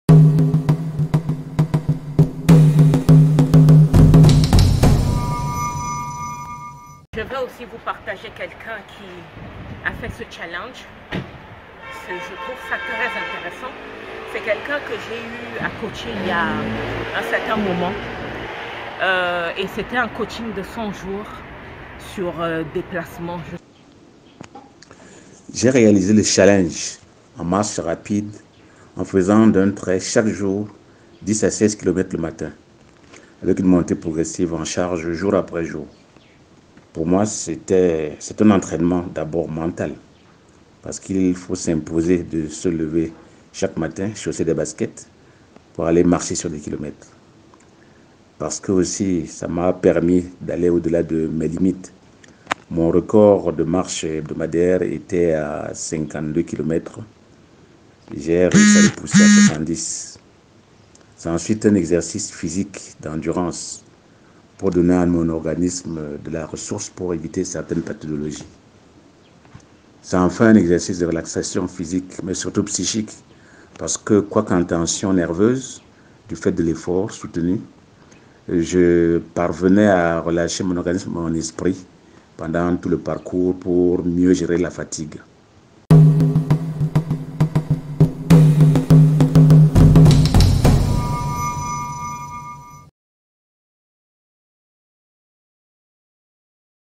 💯Mr Augustin L. 68 ans, Challenge 70 km RÉUSSI - BILAN 💪
Suivant mon example, Mr Augustin L. 68 ans a effectué le Challenge de 70km de Marche. Vous trouverez le Bilan de son expérience dans cette vidéo.

Information:
"Pour ceux qui souhaite se préparer à suivre les vidéos de WangShu, renforcer vos articulations en marchant 3 à 7 km tous les 1 à 3 Jours"
Maître DAYEM KAYYA